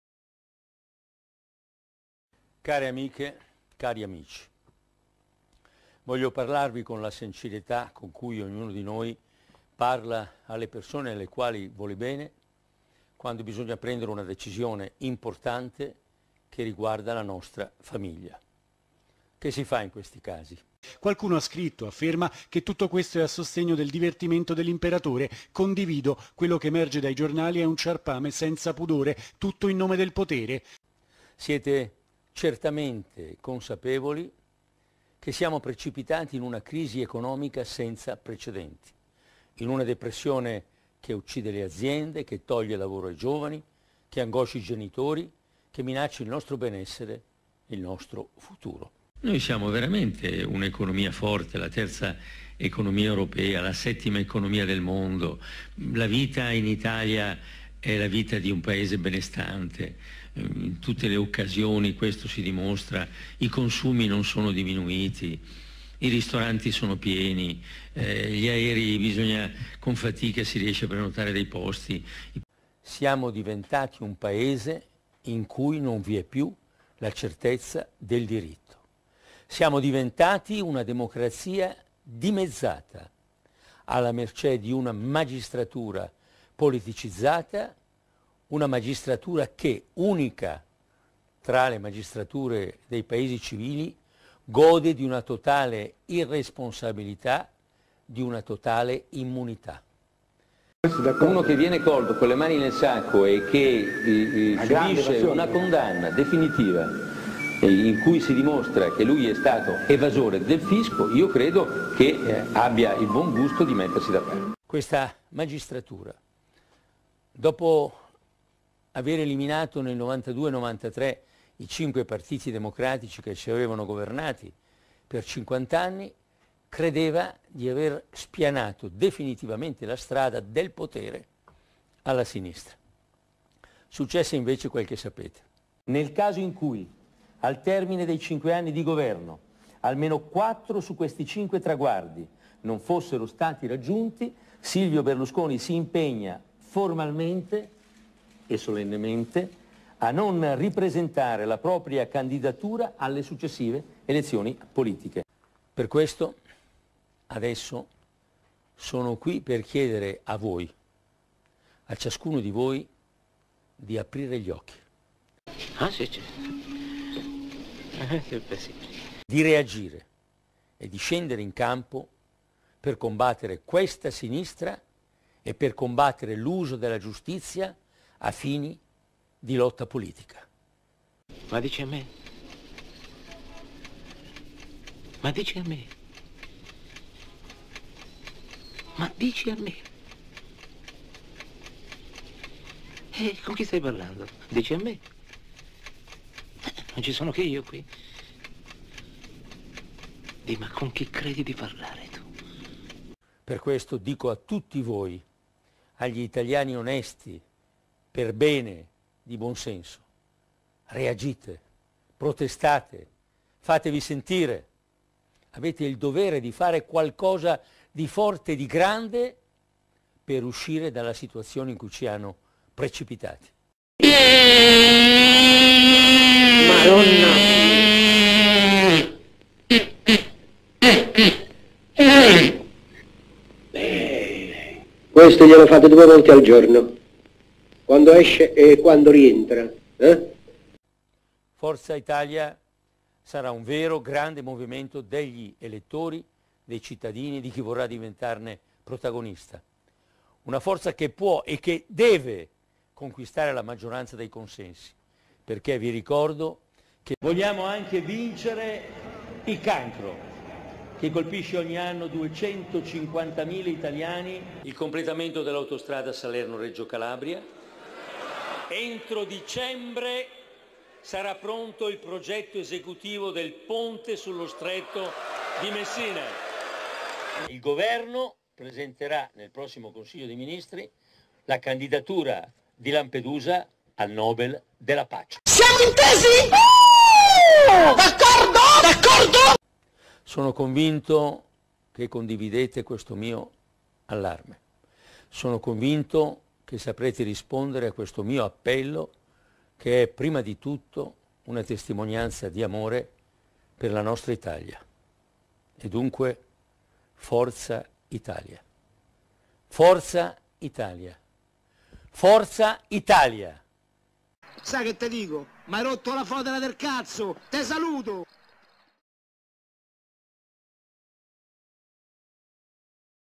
Le transaminasi di Berlusconi
Ennesima rosicata dopo la conferma della condanna al pagamento di De Benedetti.